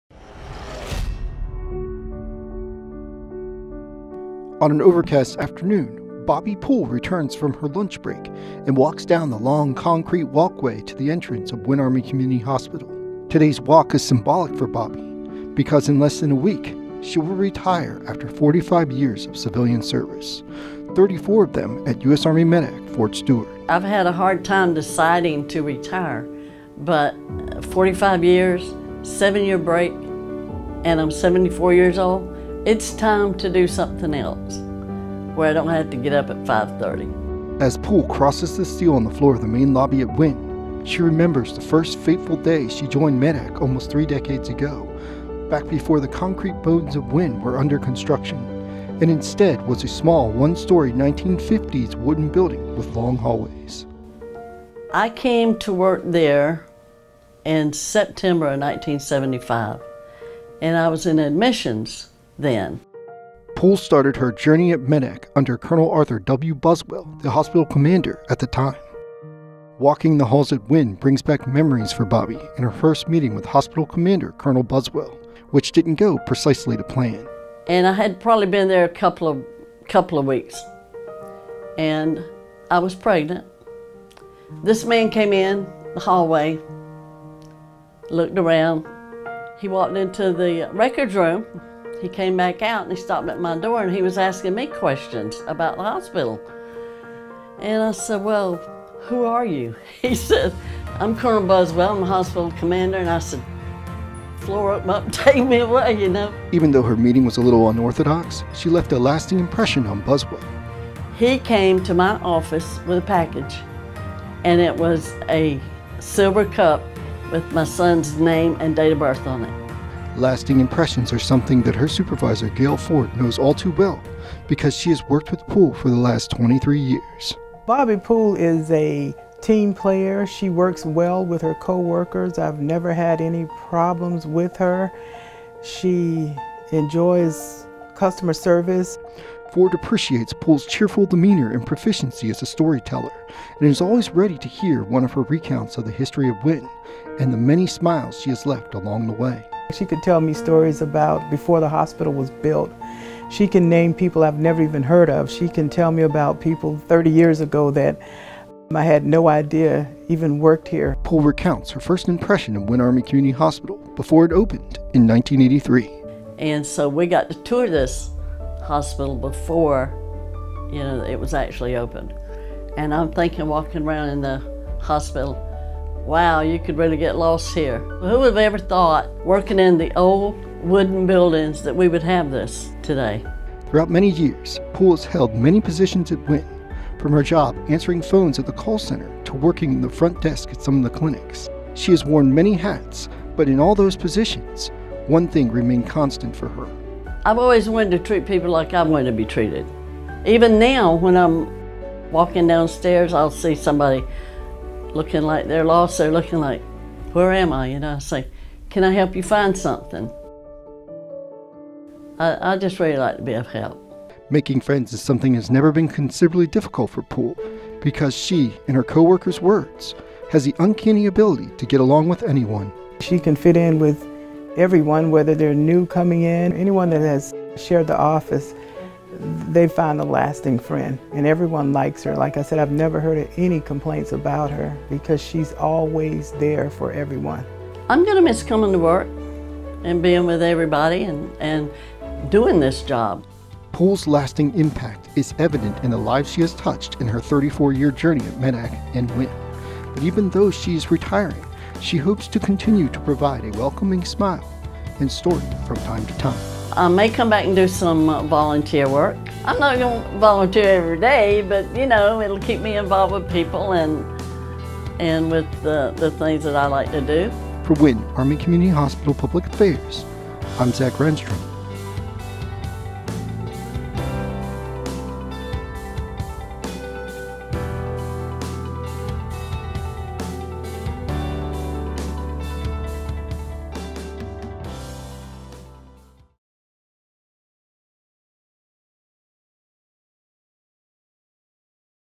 Bobbie Poole: 45 Years of Civilian Service
After 45 years working in civilian service Bobbie Poole, Referral Management Assistant at Winn Army Community Hospital is saying goodbye to her MEDDAC family and hello to retirement. Photojournalist Zach Rehnstrom has the story.

“One Team. One Purpose."